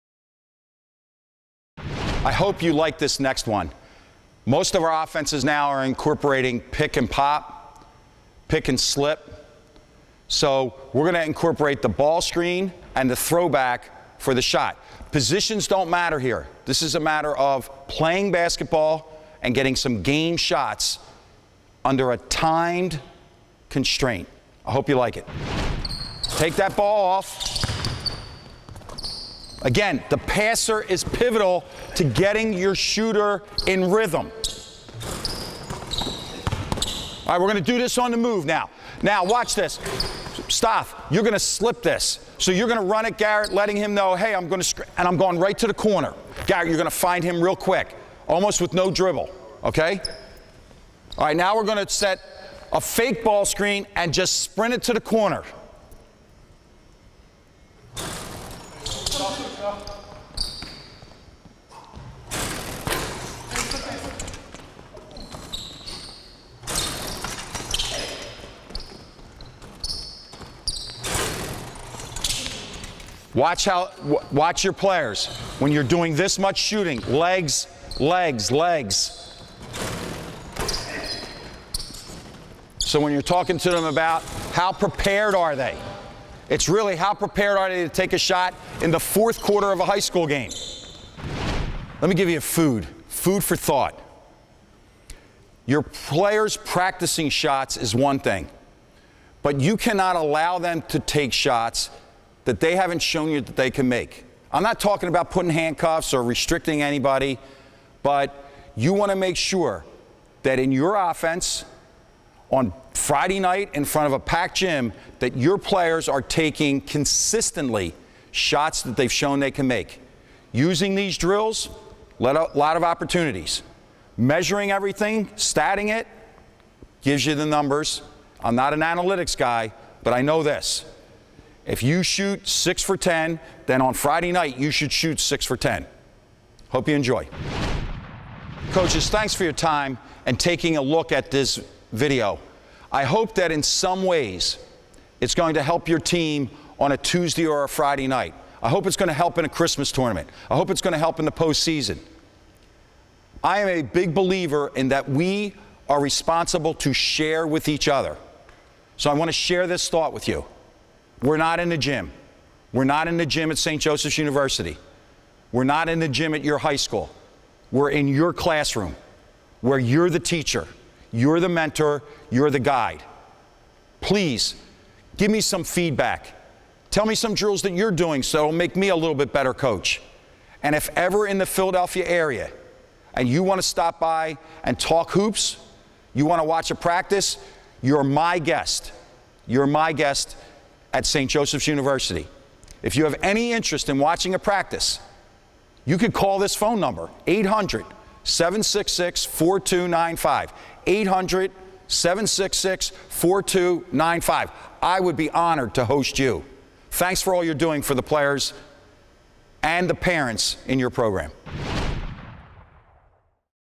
Pick & Pop drill with Phil Martelli of St Joseph
Phil Martelli shows a great basketball shooting drill using the Gun showing how to make shots off a Pick & Pop.